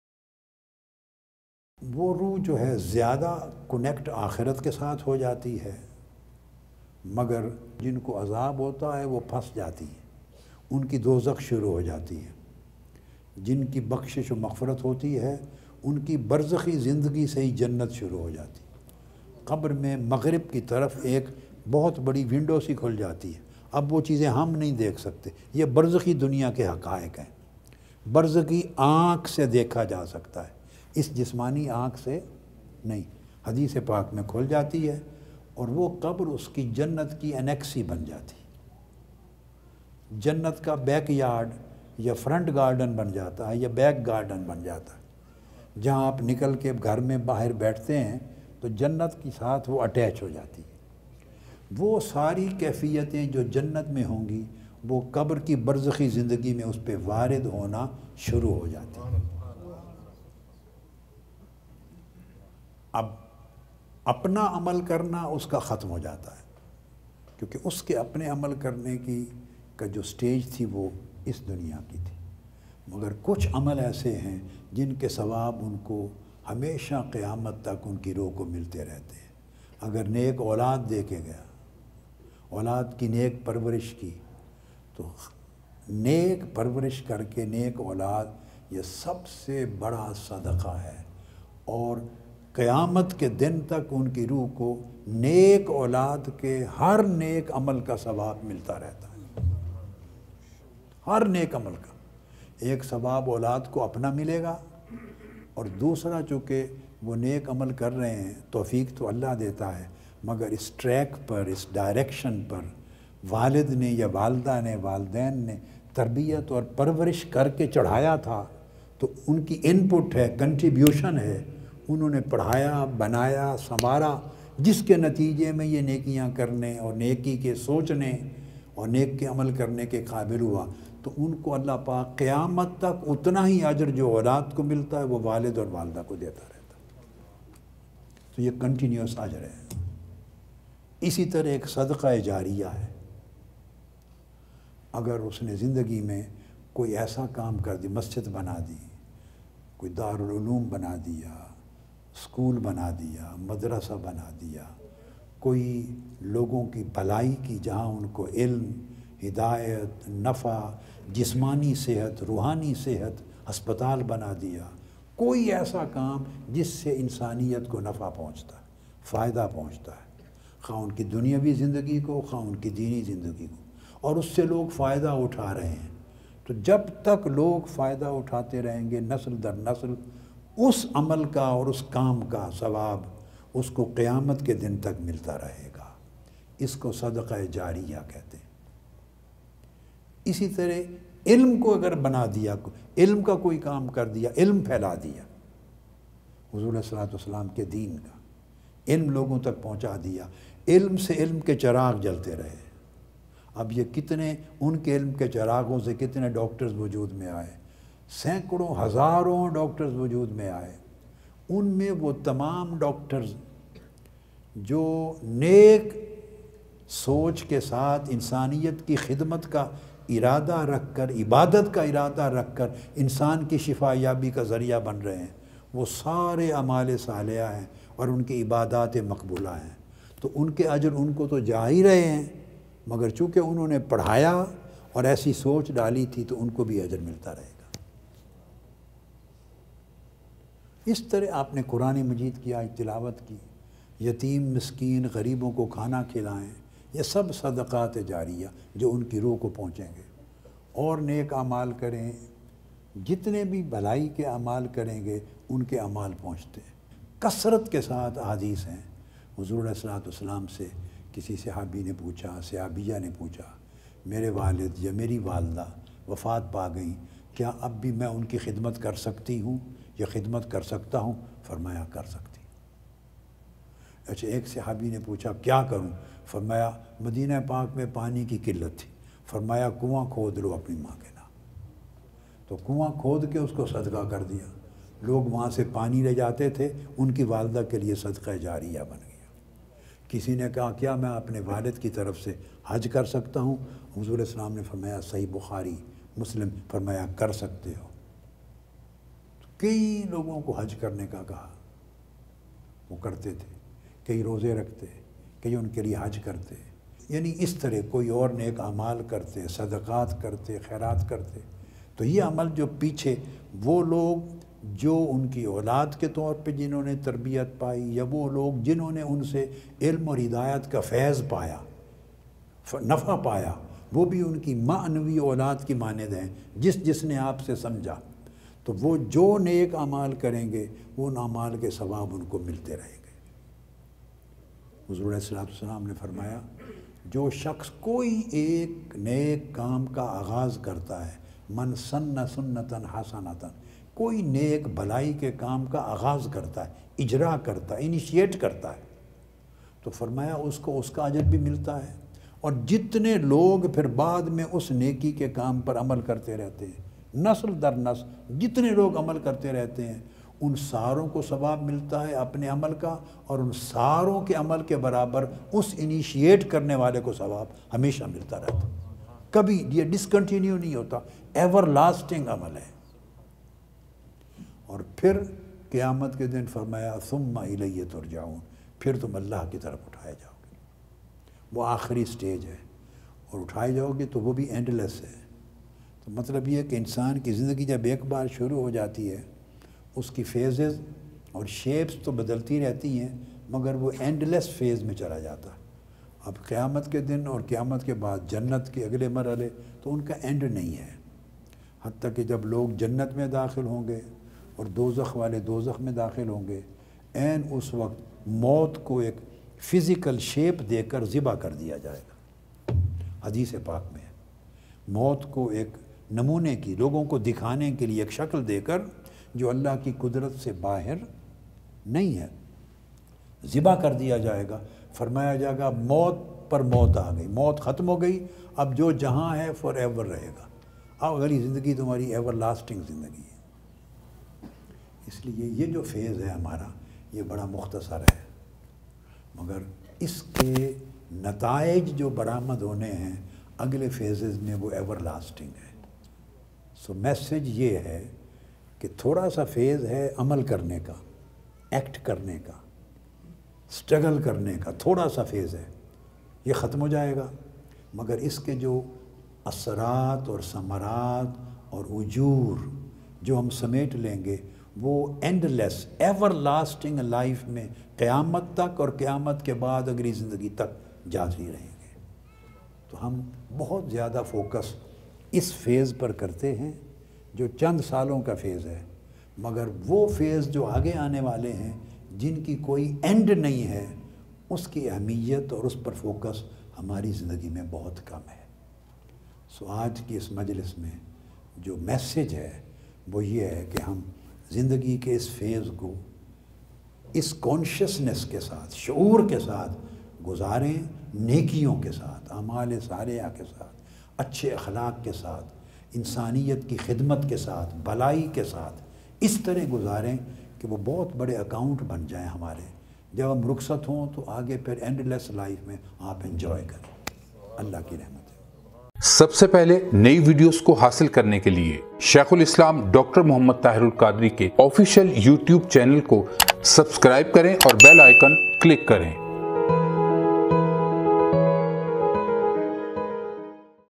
How to Turn Your Grave into a Garden of Paradise? | Shaykh-ul-Islam Dr Muhammad Tahir-ul-Qadri
اپنی قبر کو جنت کا باغ کیسے بنائیں؟ | Apni Qabar ko Jannat ka Bagh kaisy bnaen?